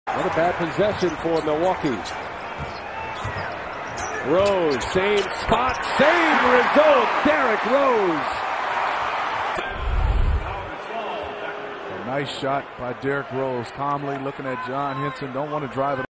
NBA, playoff 2015, Bulls vs. Bucks, Round 1, Game 5, Move 59, Derrick Rose, 2 pointer
Chicago Bulls - Milwaukee Bucks, Game 5, 27/04/2015, quarter 4
Derrick Rose, 2 pointer